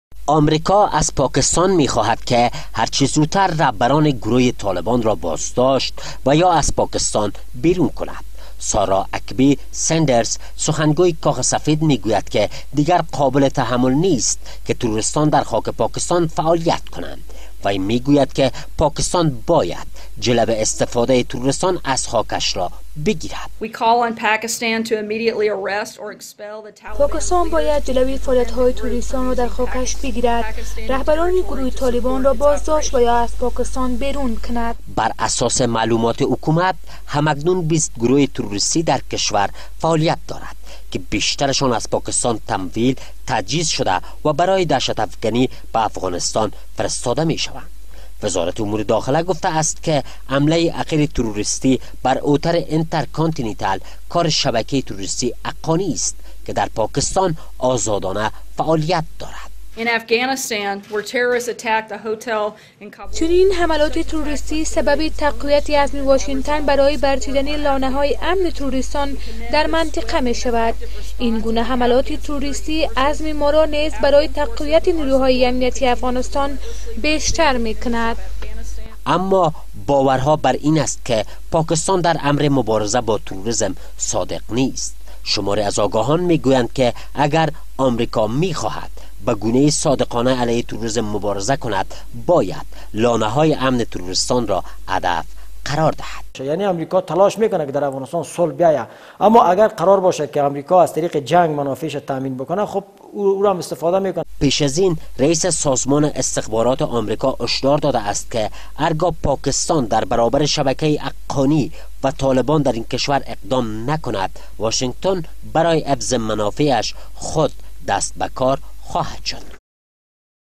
هشدار امریکا به پاکستان : در رابطه به رهبران طالبان / گزارشی از حفیظ الله امین 03 11 1396
امریکا هشدار به پاکستان گفته است رهبران گروه طالبان را در پاکستان بازداشت و یا از پاکستان اخراج کند .